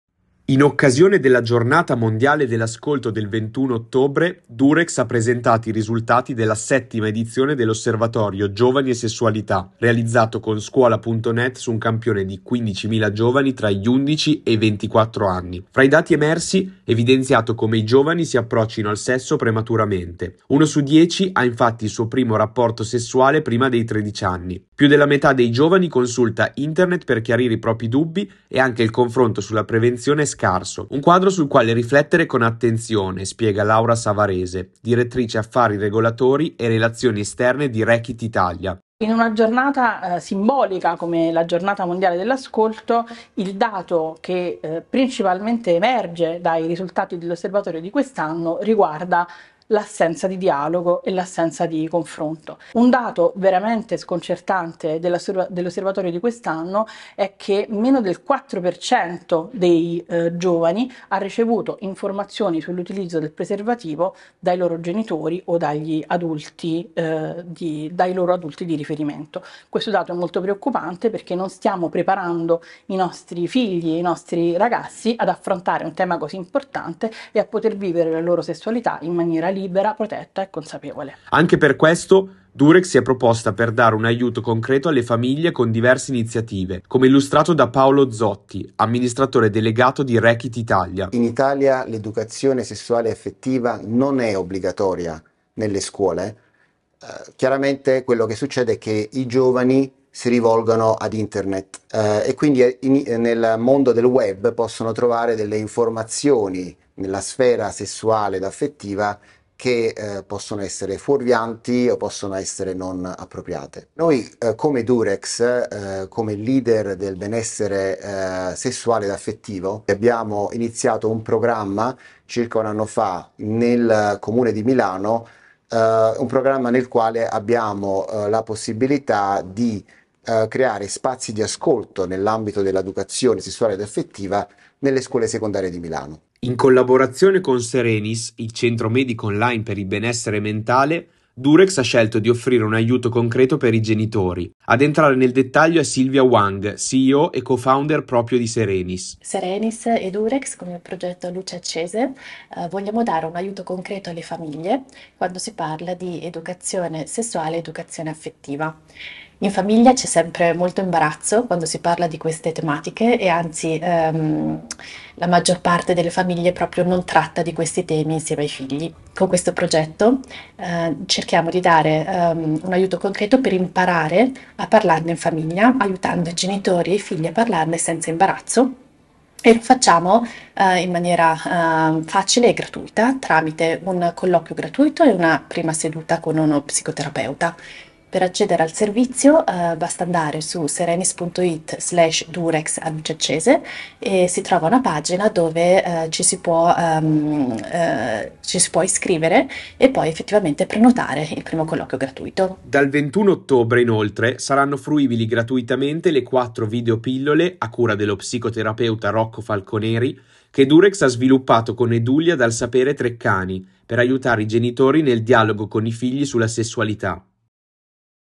Sessualità, per i giovani rapporti sempre più precoci
Giornata dell'ascolto, da Durex osservatorio Giovani e Sessualità